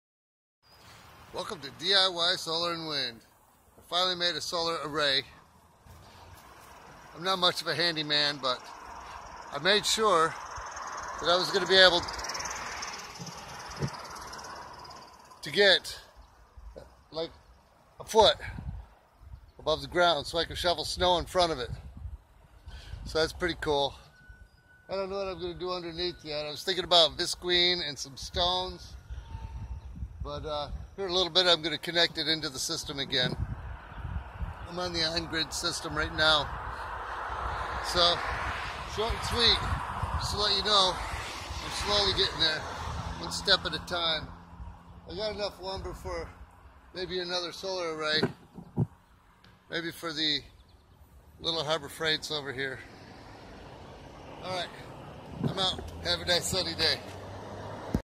Easy DIY Solar Array
DIY Solar Array. This is the first solar array that I have built. I have to build several more solar arrays before winter. I have several more solar panels that need to be on the sunny side of the house. Now that I have built this solar array I now have more room to leaned more solar panels on the side of the house. I will need to connect another grid tie inverter to the solar electrical system before I add more solar panels to the side yard.

Easy DIY Solar